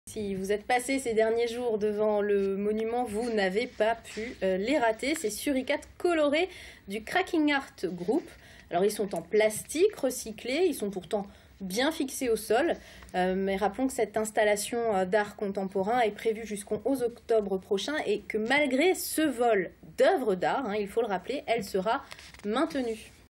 Le Mans : Vol des suricates du Cracking Art Group
Sites webs/blogs, copiez cette vidéo avec le bouton "Intégrer" !
 3 installations d'art contemporain, des suricates du Cracking Art Group au Mans, ont été dérobés.